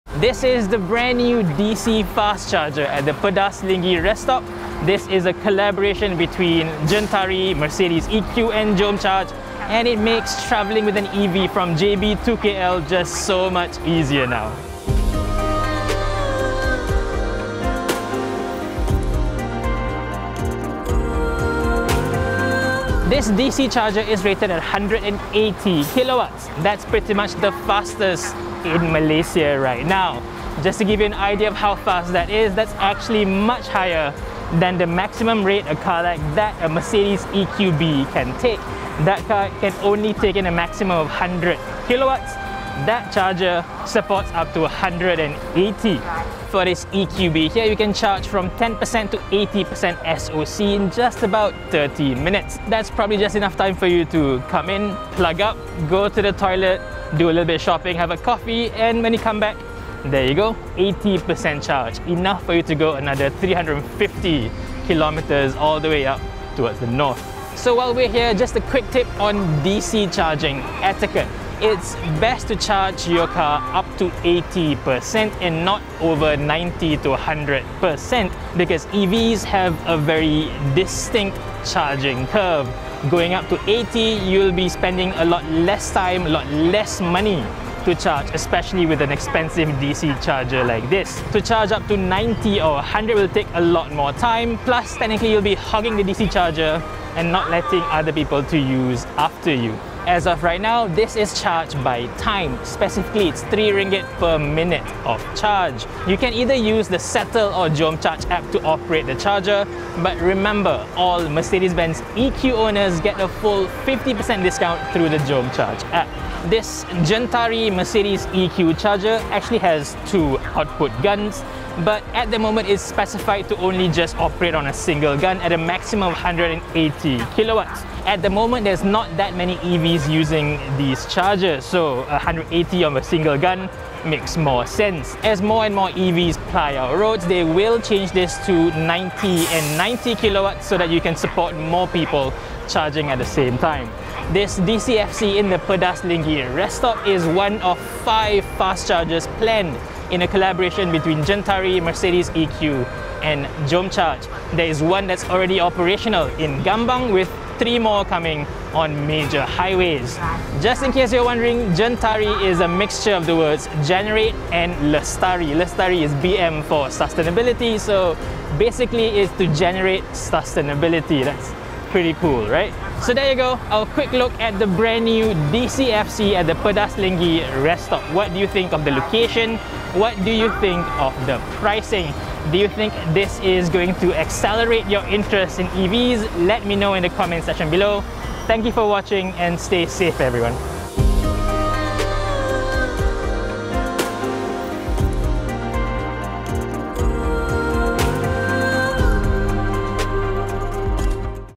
Gentari x Mercedes-EQ 180 kW DCFC - 350 km EV range for RM45, 30min!
We now have a 180 kW DC fast charger between JB and KL, enough to give you around 350 km EV range for just RM45 and 30 minutes. Is that cheap and quick enough for you?